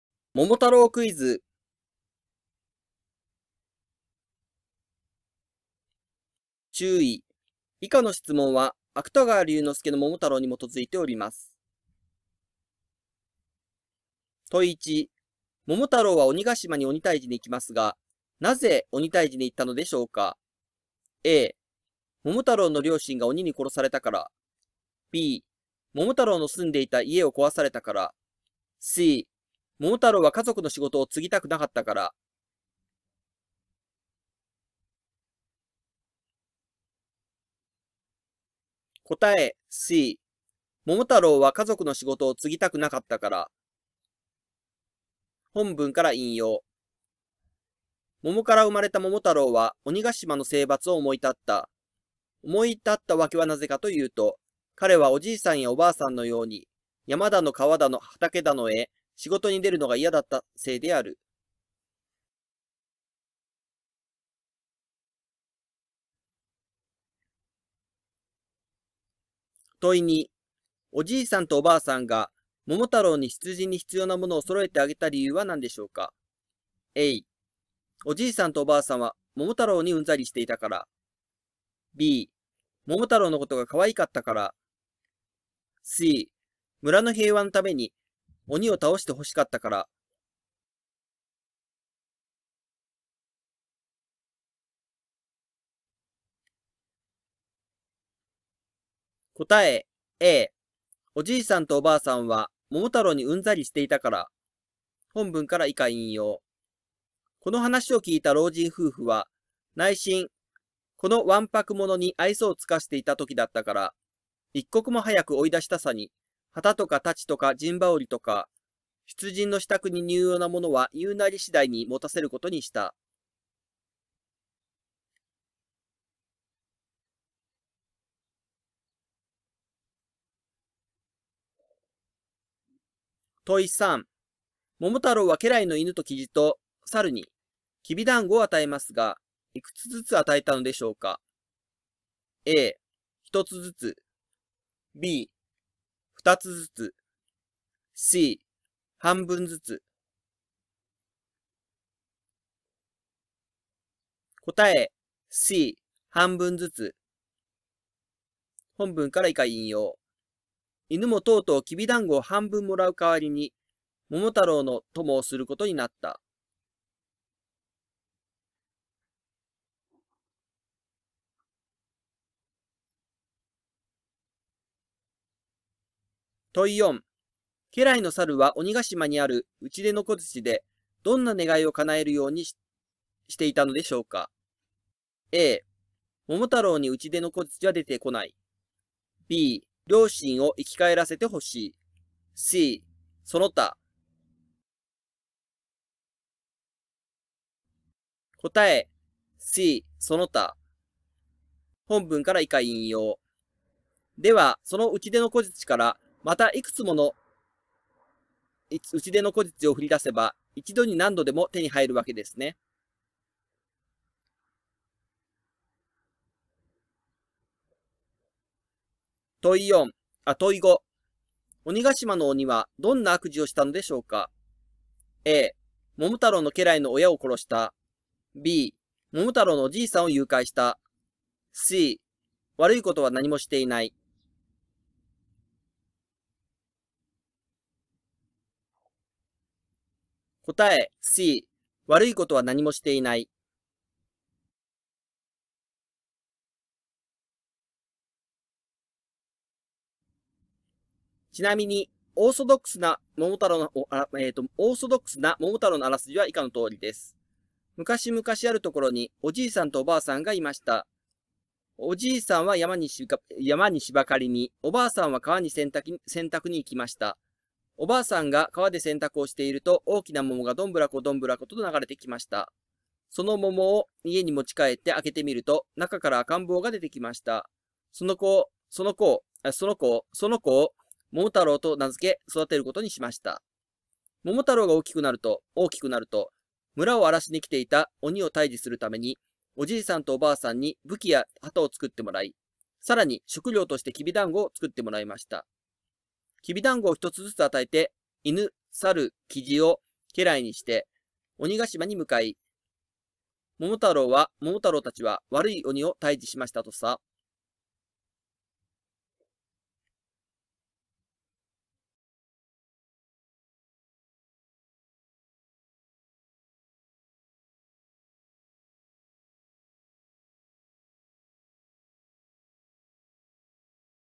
桃太郎クイズ（日本人向け）
芥川龍之介の桃太郎クイズです。日本人なら桃太郎クイズくらい余裕ですよね？この答えおかしくない？　って思われた方は是非原文を読んでみてくださいね。
昔々あるところにお爺さんとお婆さんがいました。お爺さんは山に柴刈りに、お婆さんは川に洗濯･････

there have not subtitles yet.